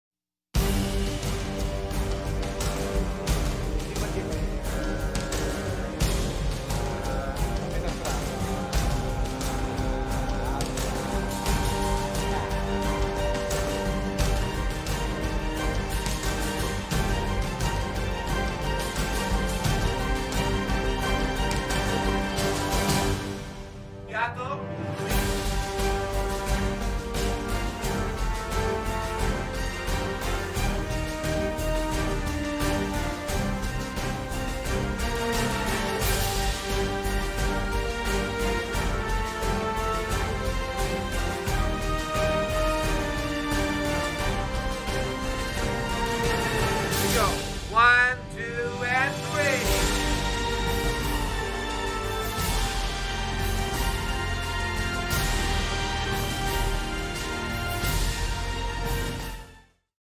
Vocal fitness, la ginnastica della lingua inglese
Steve Dapper presenta il corso di Diesse Lombardia "Vocal Fitness, la ginnastica della lingua inglese", per imparare (finalmente!) la lingua inglese.